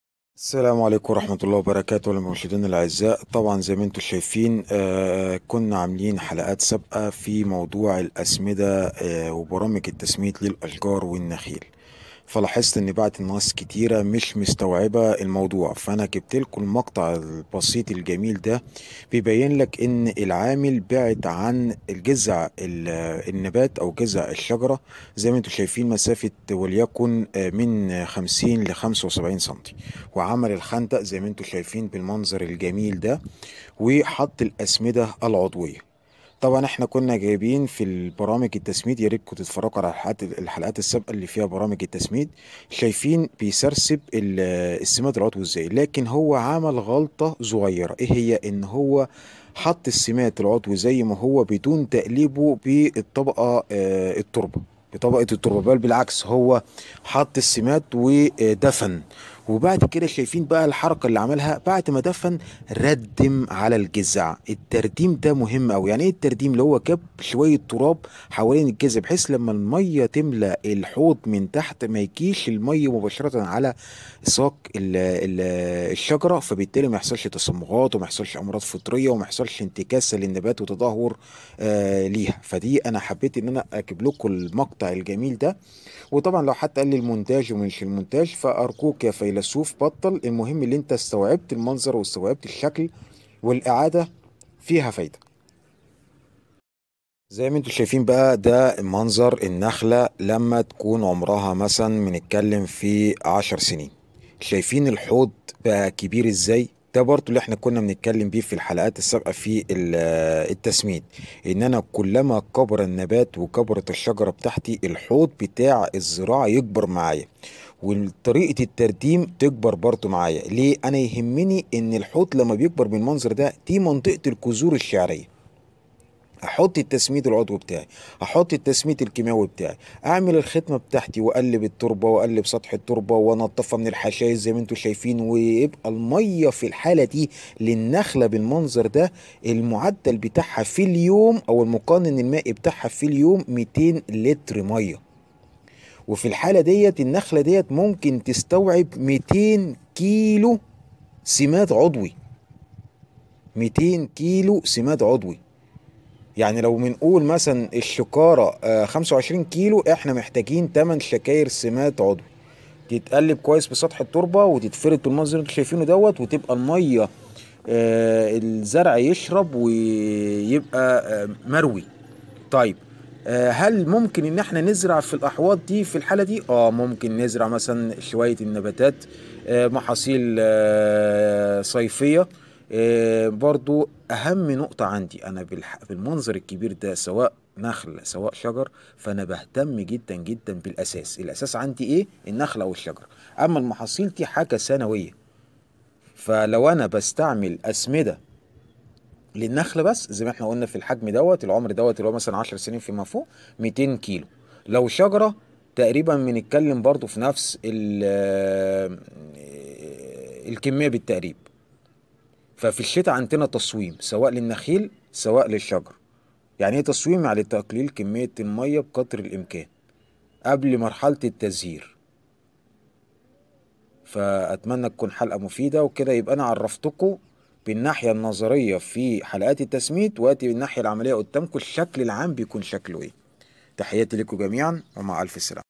طريقة تسميد العضوى والكيماوي لشجر ونخيل حلقة ٧٢١
واتس ٠٠٢٠١١٠٤٨٧٣٣٤٨لانة أسرع واشرح سؤالك بالتفصيل مع صورة واحدة فقط وشرح مفصل لسؤالك
بمقابل مادي لمزارع ومجانا للهواه